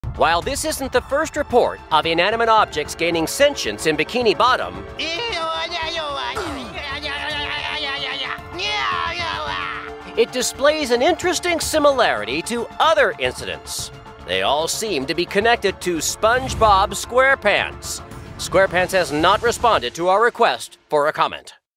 inanimate objects in bikini bottom | Bikini Bottom Inquirer
Inanimate objects gaining sentience in Bikini Bottom.. Bubble Buddy? DoodleBob? Does this really have a connection to SpongeBob..? No comment.